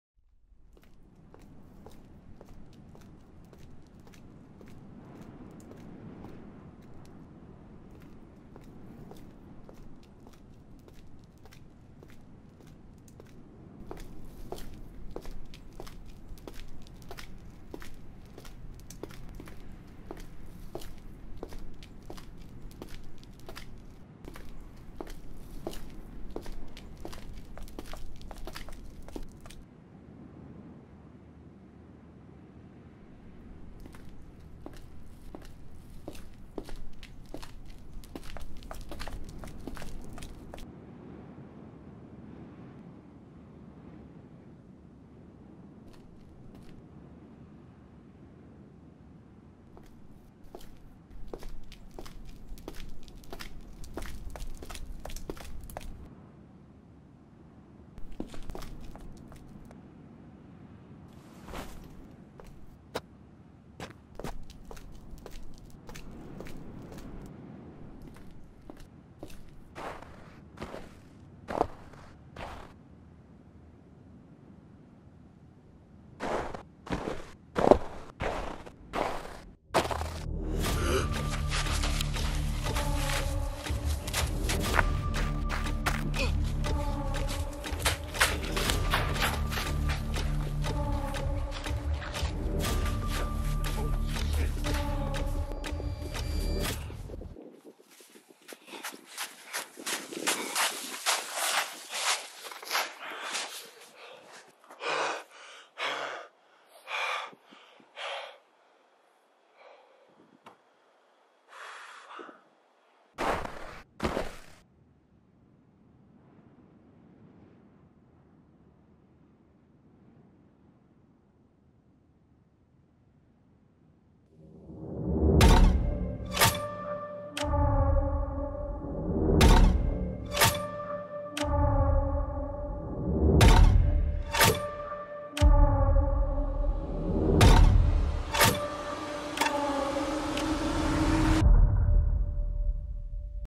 Must Have Been The Wind (Horror Short Film)
Made this in less than 2 hours with no script and no idea what we were gonna make, so this is more of an experiment than an actually short film. Overall though I think it turned out alright.